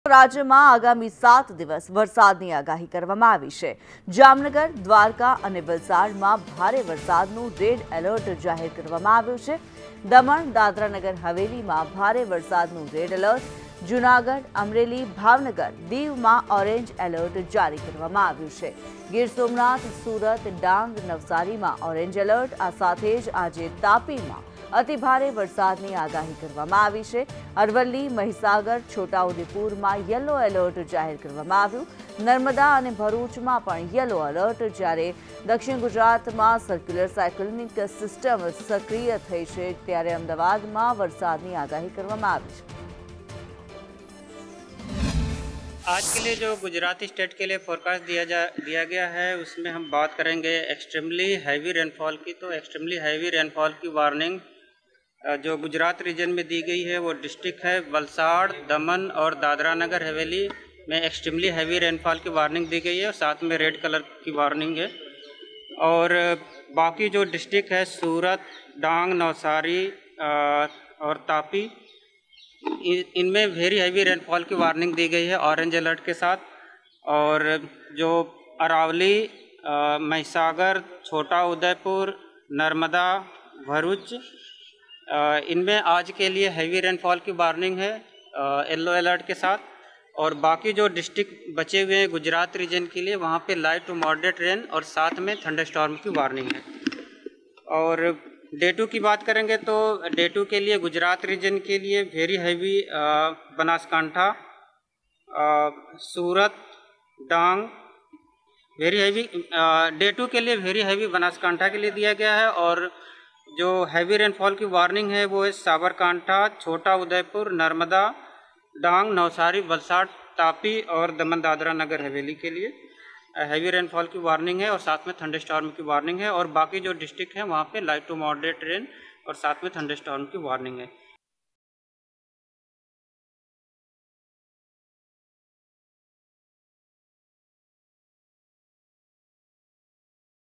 Gujarat Monsoon | ગુજરાતમાં આગામી 7 દિવસ વરસાદની આગાહી | Rain
રાજ્યમાં આગામી 7 દિવસ વરસાદની આગાહી
જામનગર, દ્વારકા, વલસાડમાં ભારે વરસાદનું રેડ એલર્ટ
દમણ, દાદરા નગર હવેલીમાં ભારે વરસાદનું રેડ એલર્ટ
જૂનાગઢ, અમરેલી, ભાવનગર, દીવમાં ઓરેન્જ એલર્ટ
ગીરસોમનાથ, સુરત, ડાંગ, નવસારીમાં ઓરેન્જ એલર્ટ
આજે તાપીમાં અતિભારે વરસાદની આગાહી
અરવલ્લી, મહીસાગર, છોટાઉદેપુરમાં યલો એલર્ટ
નર્મદા અને ભરૂચમાં પણ યલો એલર્ટ
દક્ષિણ ગુજરાતમાં સર્ક્યુલર સાયકલોનિક સિસ્ટમ સક્રિય
આજે અમદાવાદમાં વરસાદની આગાહી
બે દિવસ માછીમારોને દરિયો ન ખેડવા સૂચના